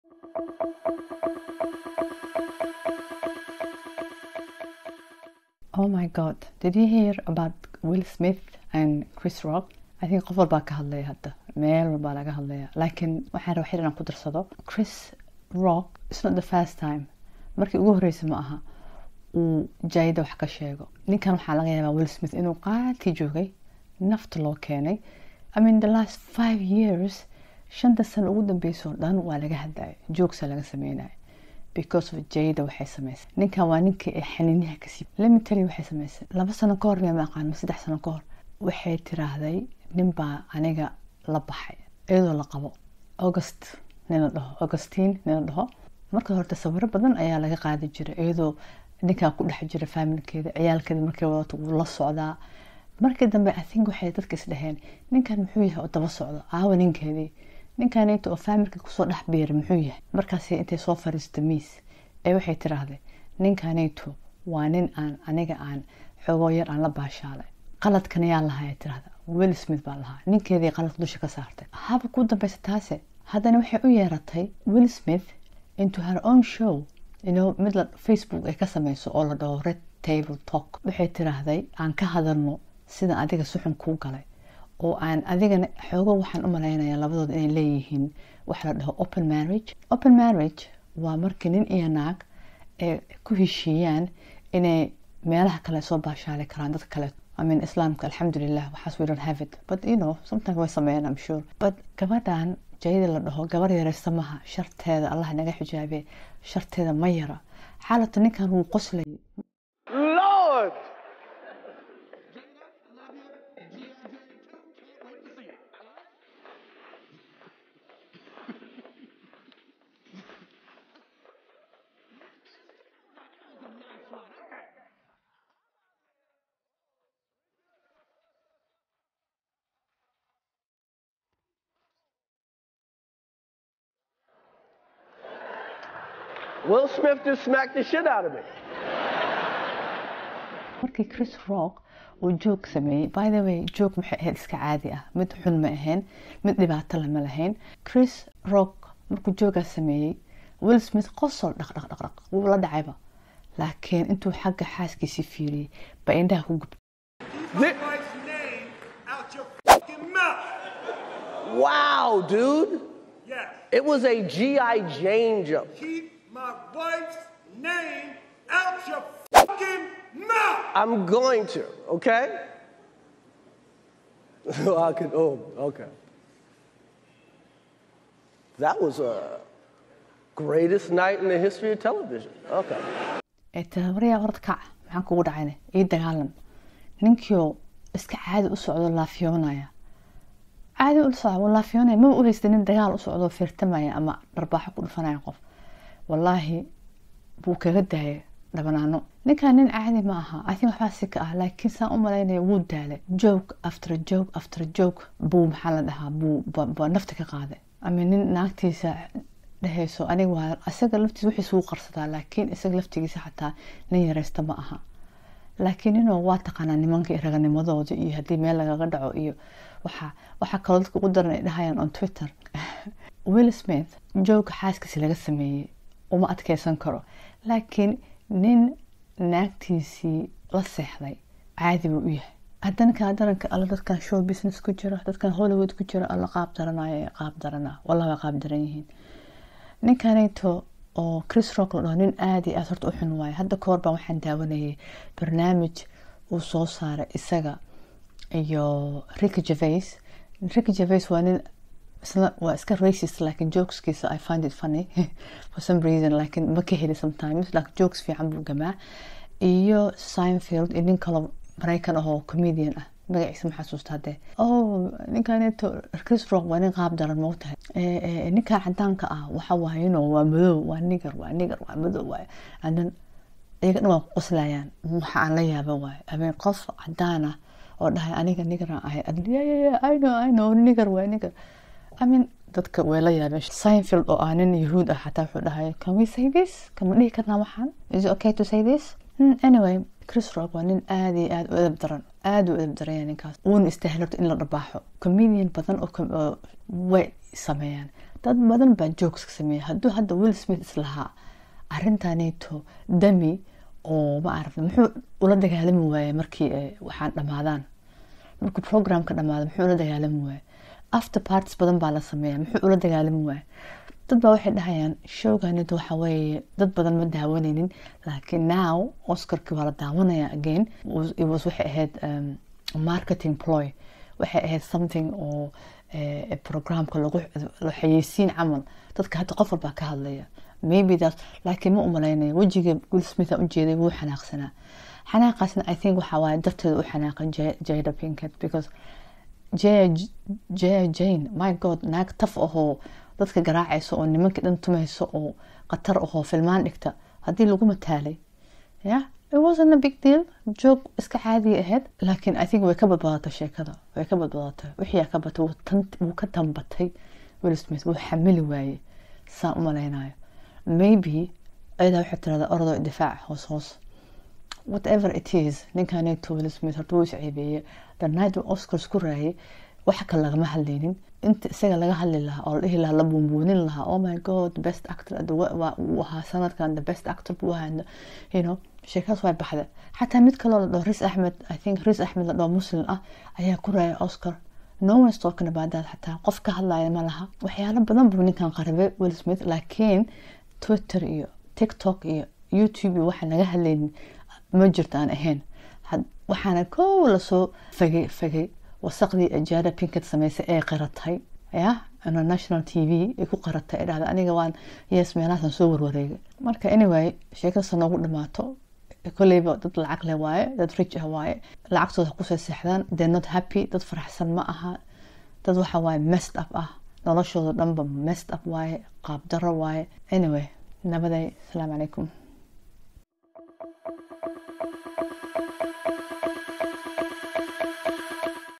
WILL SMITH TII UGU DANBEYSAY AYEY JADA PINKET GAARSIISAY
Wuu xamili waayey daraamaheedii iyo iyadoo meel uga dhacdey raganimadiisii.